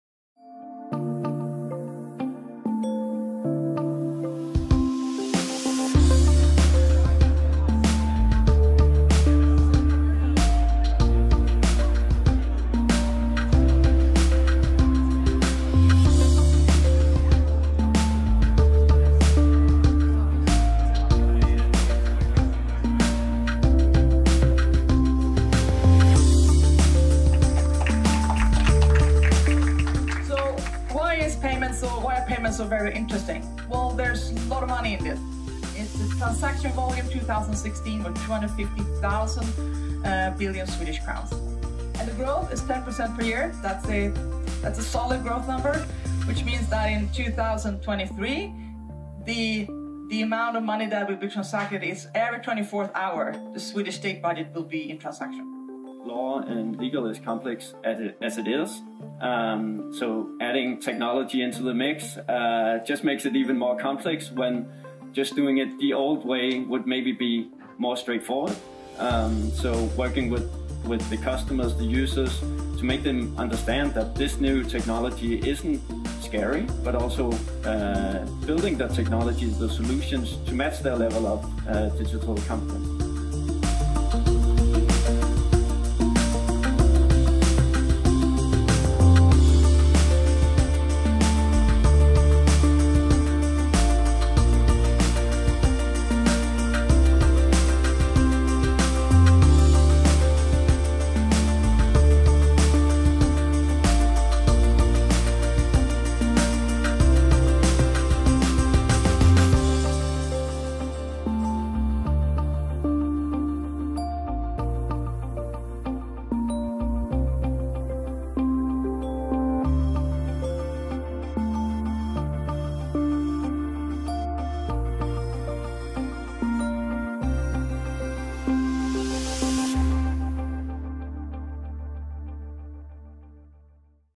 Nordic Legal Tech Day 2019
Under Nordic Legal Tech Day 2019 samlades ett stort antal innovationsdrivna företag, från Sverige och övriga världen, för att visa upp sina produkter, knyta kontakter och dela kunskap med varandra.